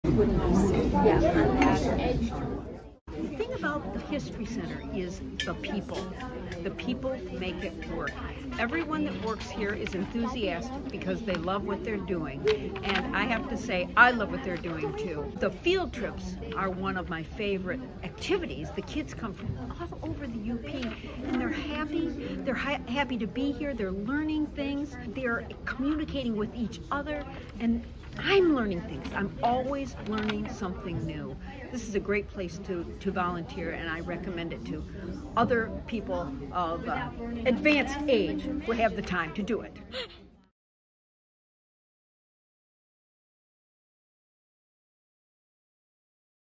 Volunteer Recruitment | Sue VanderVeen
~The Marquette Regional History Center wants YOU!~
We are always looking for more volunteer help with our field trips. If you are a retired educator, a student who needs volunteer hours, or a person who likes to get involved with the community...then this volunteer job is for you.
Our Education Department Volunteer group is welcoming and fun! We drink coffee, eat pastries, and always have good conversation during our time together. We recreate historical characters for students, give tours of the exhibits, get immersed in a flashback to the Fur Trade and more.
If you're interested in joining us, please do not hesitate!

X: @/ MarquetteMuseum